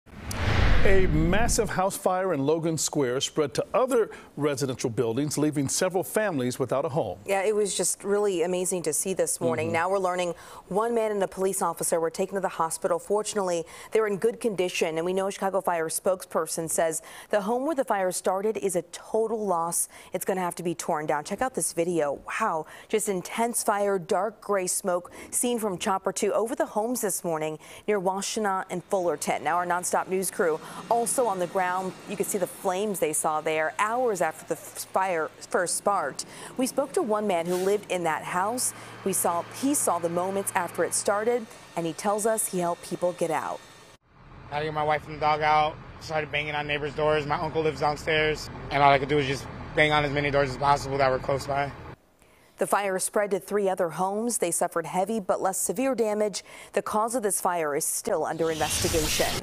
Logan Square fire destroys 1 house, damages 3 others
The Chicago Fire Department confirmed the fire broke out at 2444 N. Washtenaw Ave. CFD said that building will be a total loss after the rear collapsed.